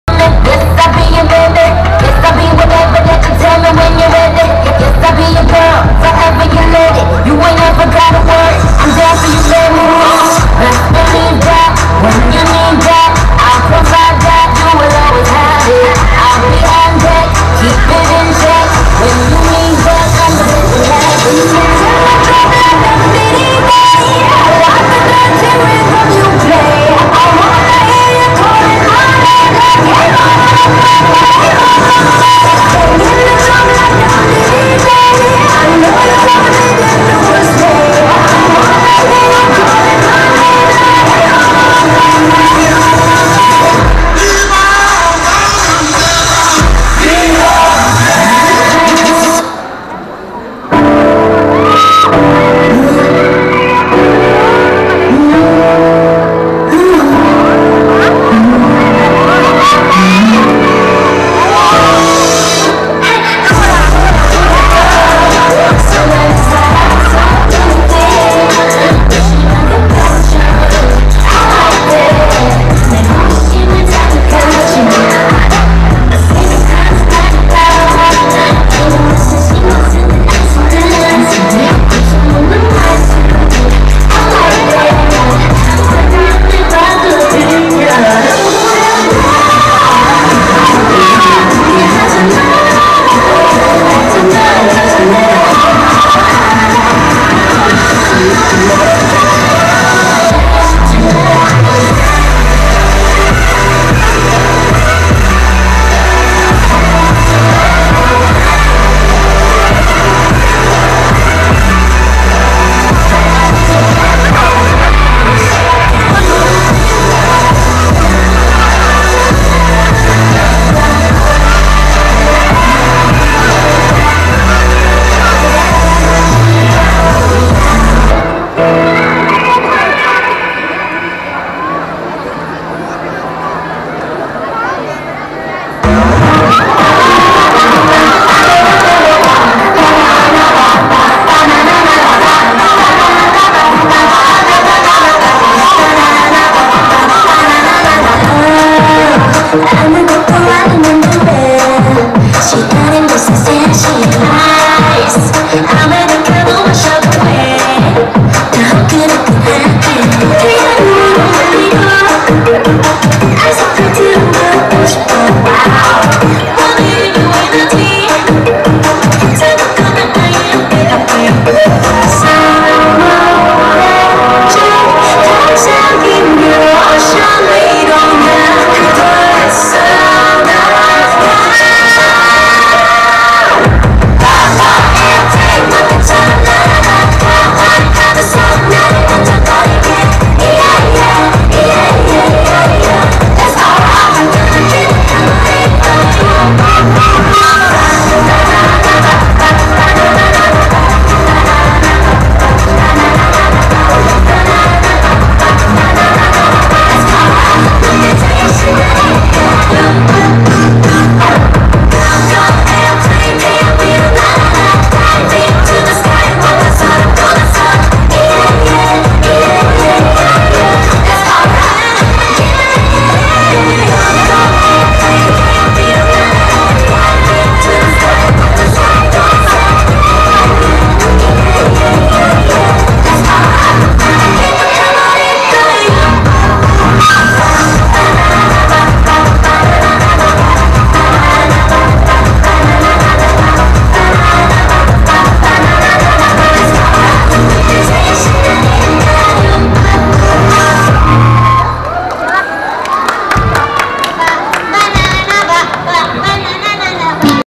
Blood Diamond Dance Cover Red Velvet at KPOP is Your Life Depok Town Square 151219
Group : Blood Diamond
Cover : Red Velvet
Event : KPOP is Your Life at Depok Town Square by MDF Entertainment